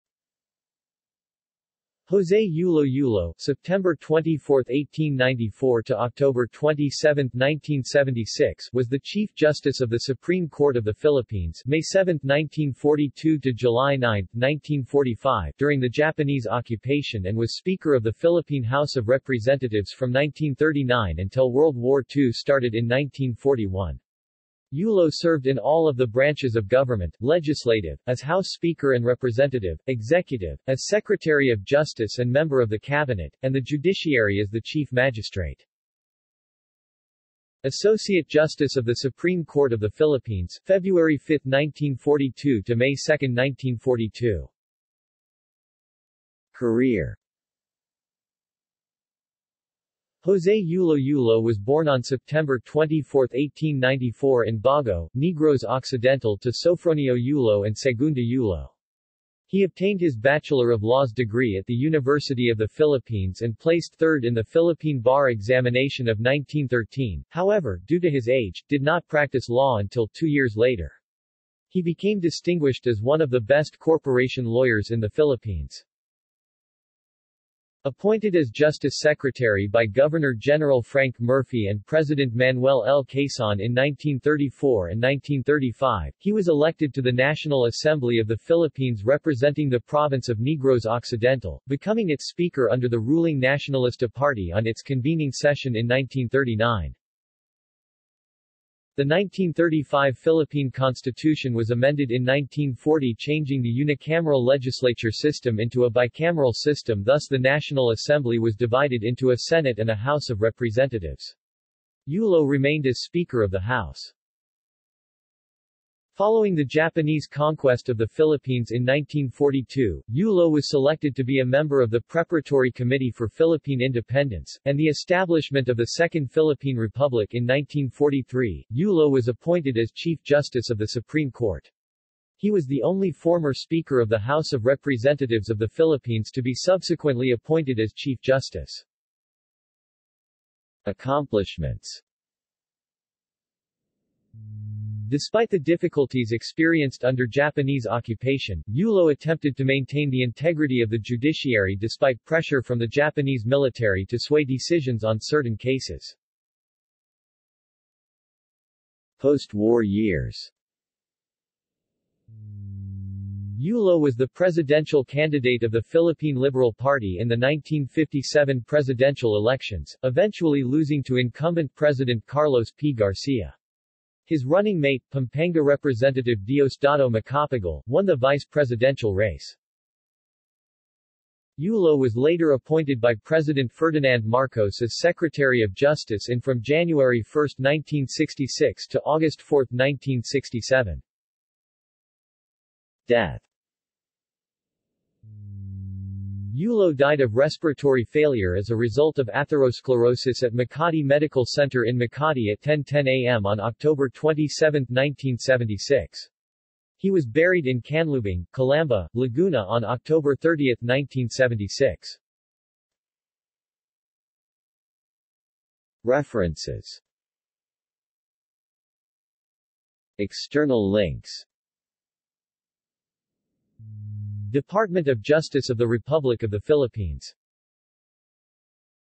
José Yulo | Wikipedia audio article
This is an audio version of the Wikipedia Article:
José Yulo

SUMMARY
José Yulo Yulo (September 24, 1894 – October 27, 1976) was the Chief Justice of the Supreme Court of the Philippines (May 7, 1942 – July 9, 1945) during the Japanese Occupation and was Speaker of the Philippine House of Representatives from 1939 until World War II started in 1941. Yulo served in all of the branches of government: Legislative, as House Speaker and Representative; Executive, as Secretary of Justice and member of the Cabinet; and the Judiciary as the Chief Magistrate.
Associate Justice of the Supreme Court of the Philippines (February 5, 1942 – May 2, 1942)